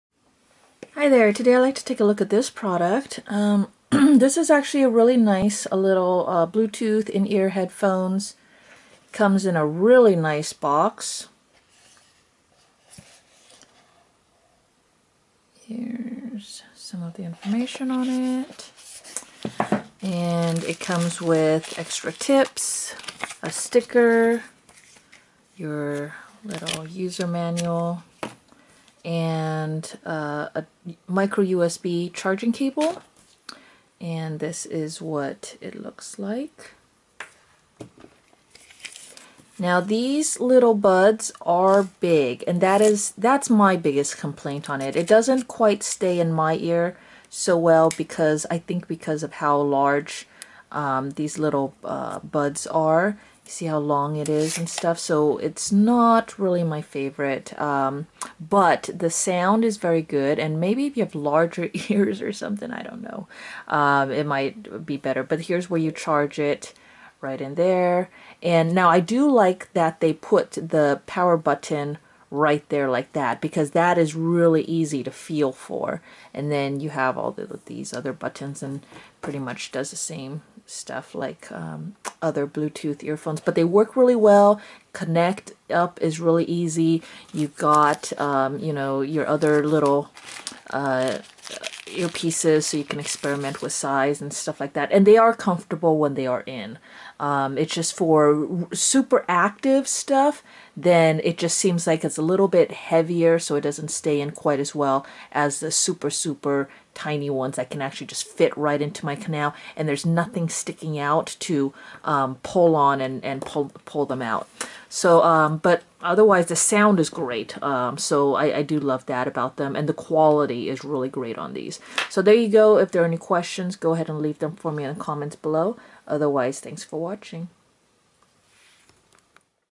1MORE Bluetooth Sport In Ear Headphones - 2018 iBFree
Highlights of my Video Review:

1.  Even though I think these are pretty decent earbuds, they are not the most comfortable and don't stay in well in my ears while being very active, so they don't work well for me.
2.  For really good sound quality, it seems you gotta look in the $200.00+ dollar range.  Anything under $100.00, I consider to be "budget" earbuds and I don't expect too much from them.  I think the sound quality of these are great for the price.
3.  Great packaging, you get extra tips and micro-usb charging cable.